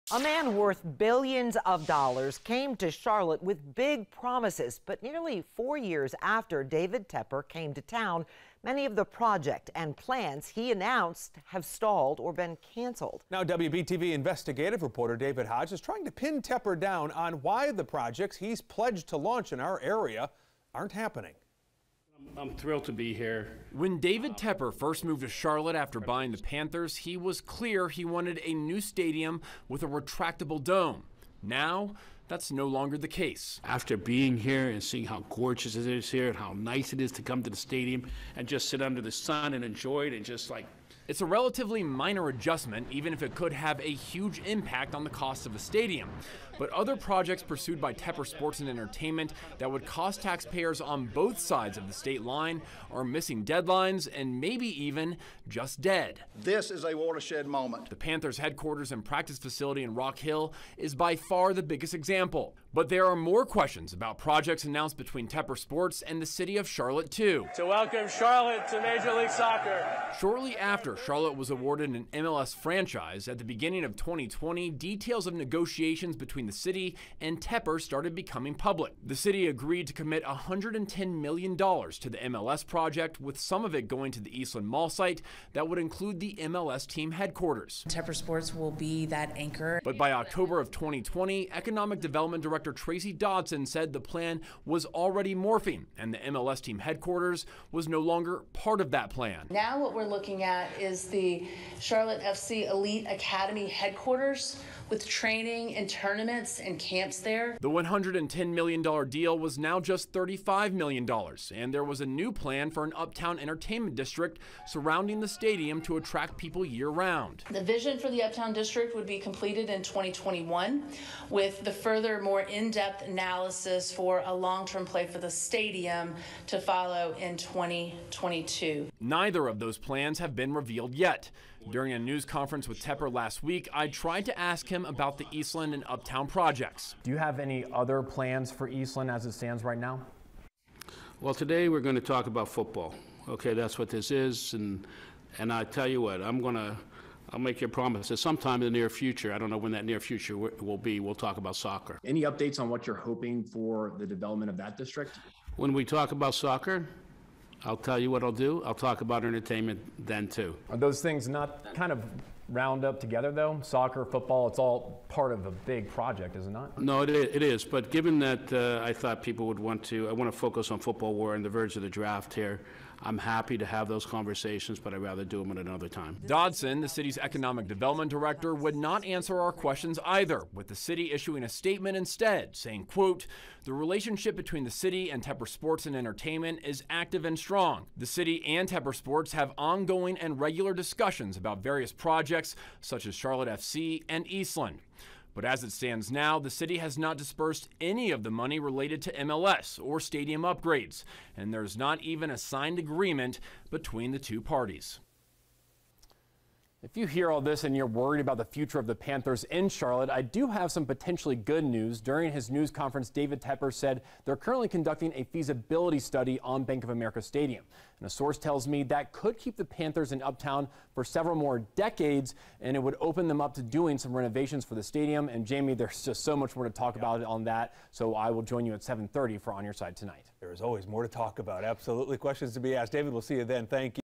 Panthers Owner Dodges Questions On Eastland, Rock Hill And Uptown District
Since his very first days in Charlotte, Panthers and Charlotte FC owner David Tepper has been described as a straight shooter. But a WBTV investigation and review of public-private partnerships with Tepper Sports & Entertainment raises questions about how often the billionaire’s target is shifting.